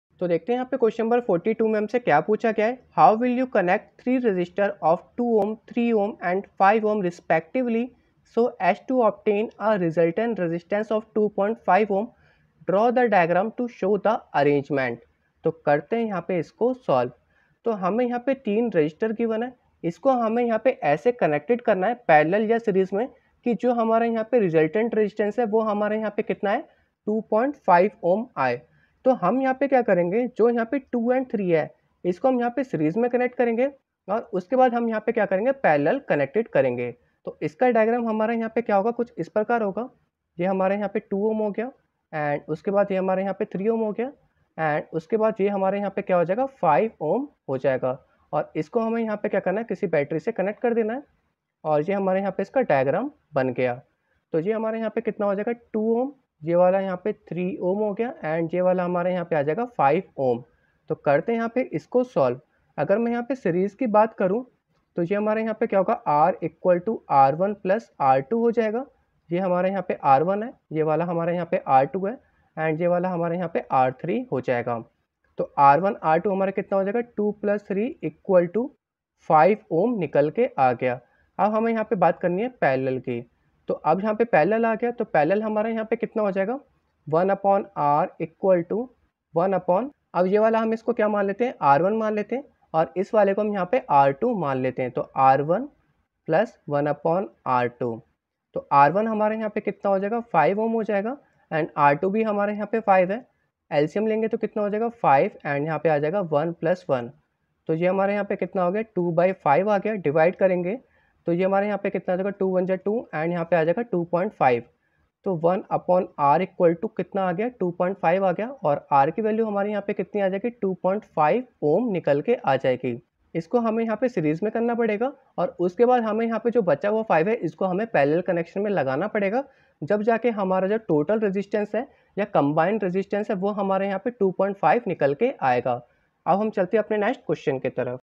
How will you connect three resistors of 2 Ω 3 Ω and 5 Ω respectively so as to obtain a resultant
Q.42 How will you connect three resistors of 2 Ω, 3 Ω and 5 Ω respectively so as to obtain a resultant resistance of 2.5 Ω ? Draw the diagram to show the arrangement.